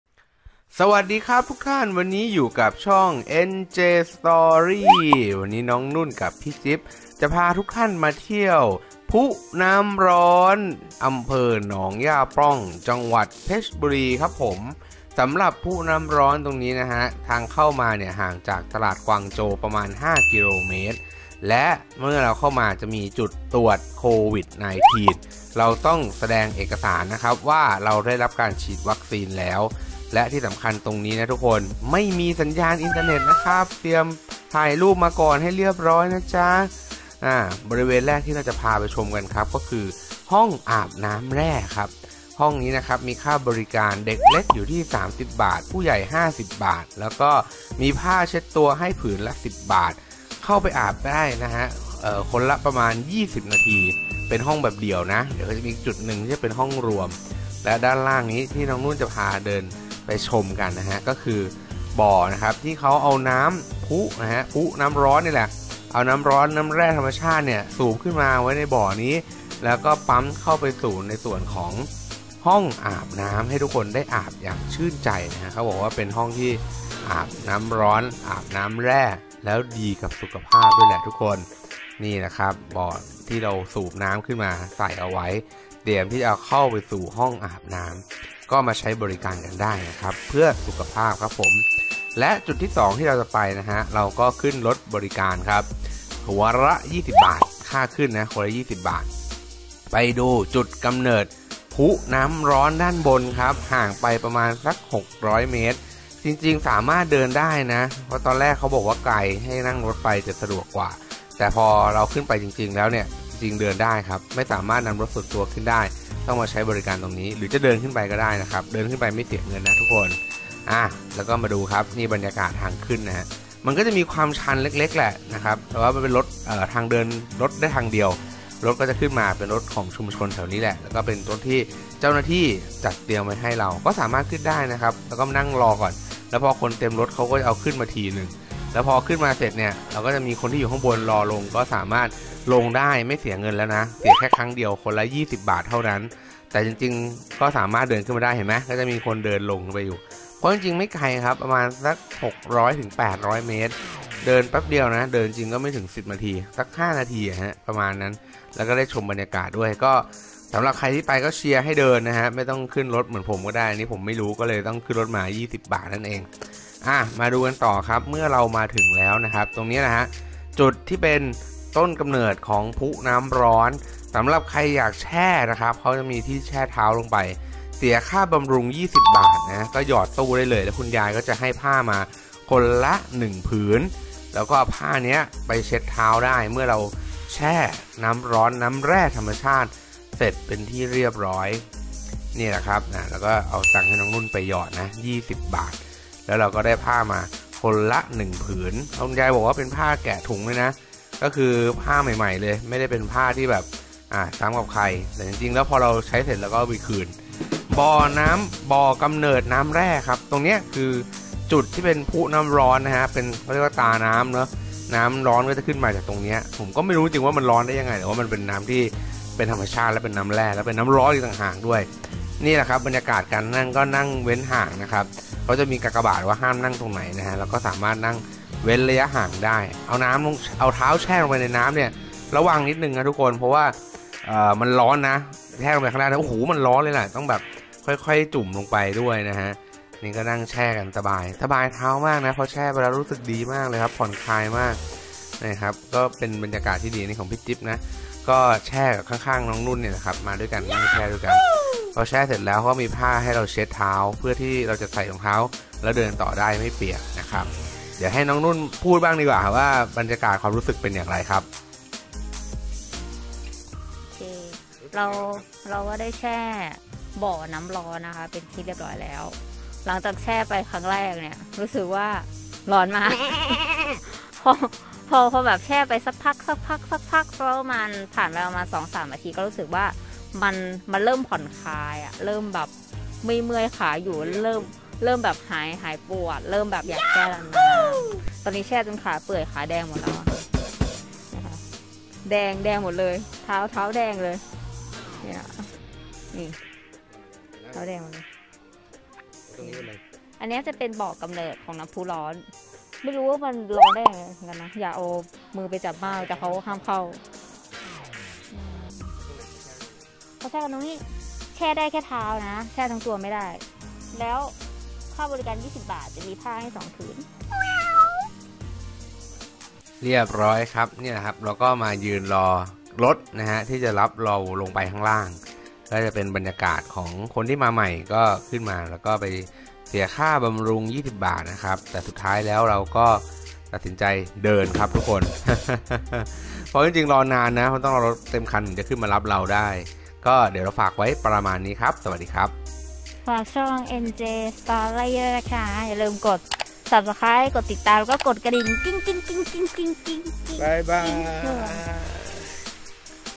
บ้านพุน้ำร้อน อำเภอหนองหญ้าปล้อง จังหวัดเพชรบุรี | NJ Story
สวัสดีครับทุกท่าน วันนี้ NJ Story จะพาทุกท่านไปเที่ยว บ้านพุน้ำร้อน อำเภอหนองหญ้าปล้อง จังหวัดเพชรบุรี จะสวยงามและแชร์น้ำร้อนดีขนาดไหน ไปรับชมกันครับ